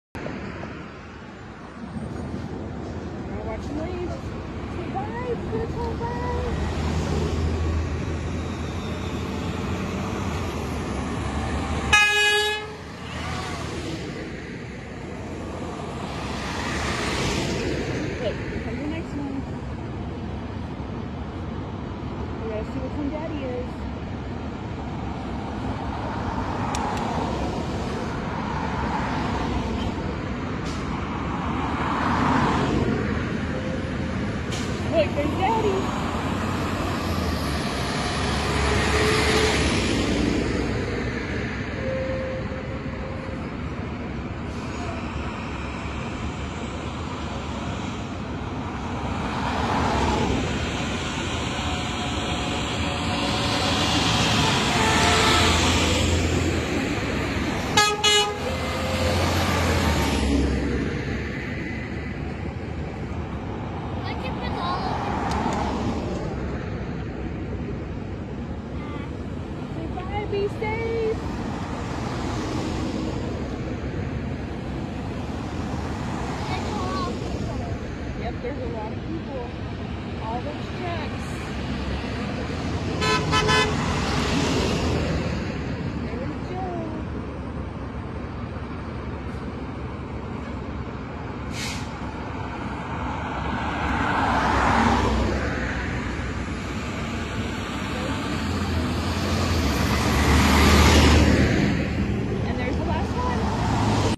San Angelo, Texas, Linemen Head To Florida To Help Hurricane Ian Victims
Praying for all the linemen that are in/heading to the Florida area. While people run from the storm y’all run to it. I pray for y'alls safety and I pray for the families y’all leave behind to help others.